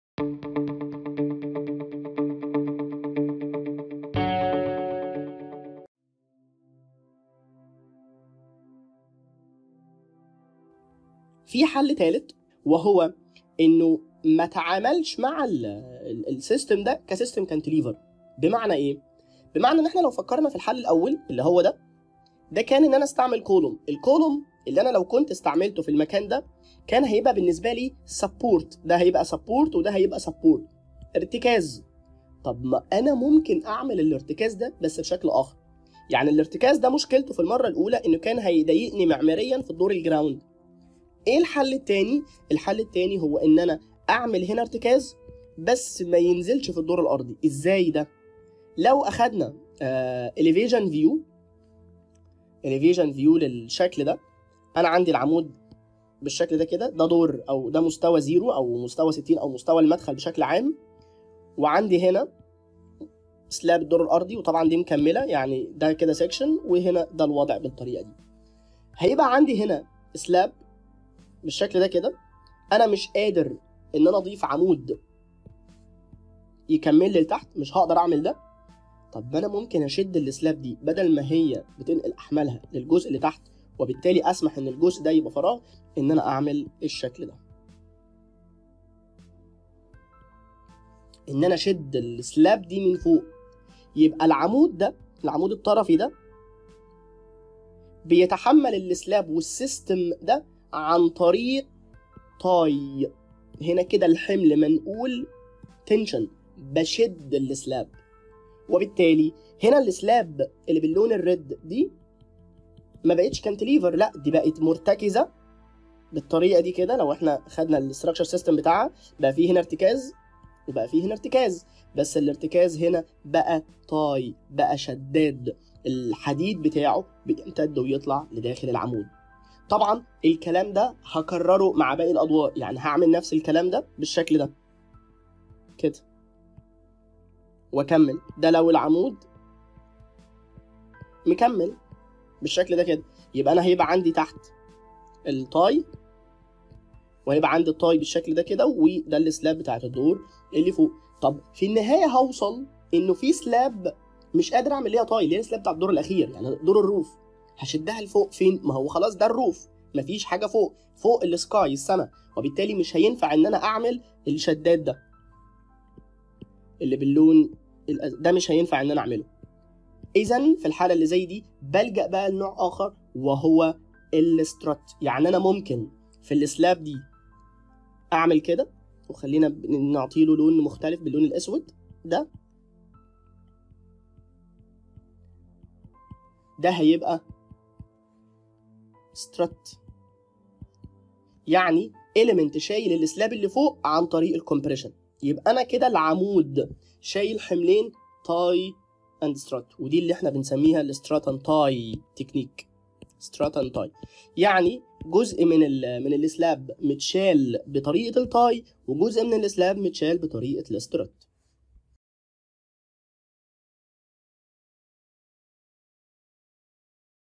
Full Projects Designs -08-04- Strut & Tie - Solution (2) for cantilever case study
هل في حد أقصي لطول الكابولي ؟
الحقيقة السؤال ده عام شوية ، وبيتسأل دايما واحنا بنوضح تفاصيل الكوابيل في أنواع مختلفة من المنشآت.

أحد الحلول الجوهرية لحل الترخيم في الكوابيل الكبيرة (نسبياً) هو استخدام strut and tie RC elements

في شابتر 7 من كورس المشاريع الكاملة STR07-FULL PROJECTS DESIGNS ، تناولنا شابتر كامل يتناول 3 حالات دراسة للنوع ده من الأفكار الانشائية اللي ممكن تحل لك موضوع الترخيم ده بشكل جوهري ، 3 حالات دراسة ل 3 مشاريع مختلفة بحيث نشوف كيفية استخدامها وتصميمها و نمذجتها.

ميزة الحل ده انه مفيد إنشائيا بحيث ما يبقاش الكابولي كله طاير ، ولكن يبقي مرتكز في نهايته
و كمان يبقي مخفي معمارياً .

قريباً هنعلن عن كل تفاصيل الكورس الدرسم ..